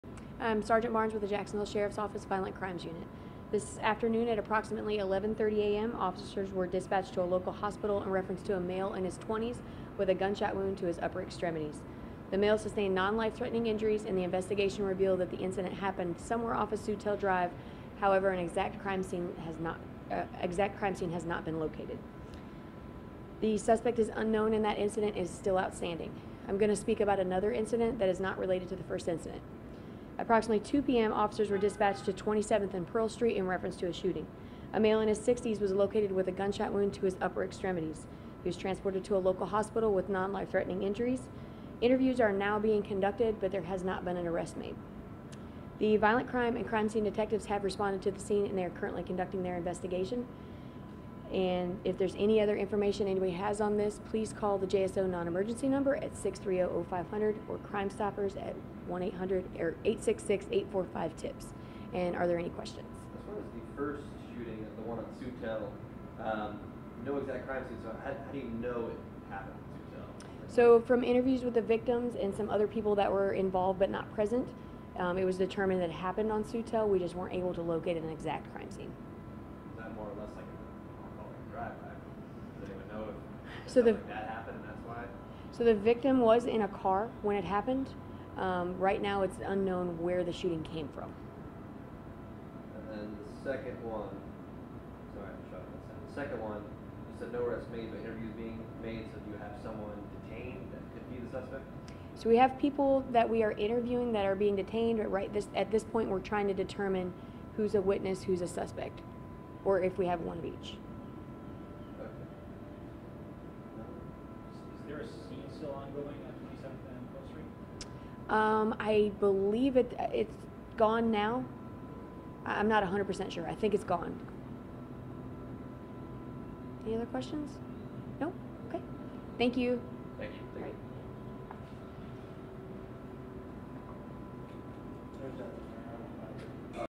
JSO gives details about two separate shootings
Both victims are expected to be OK.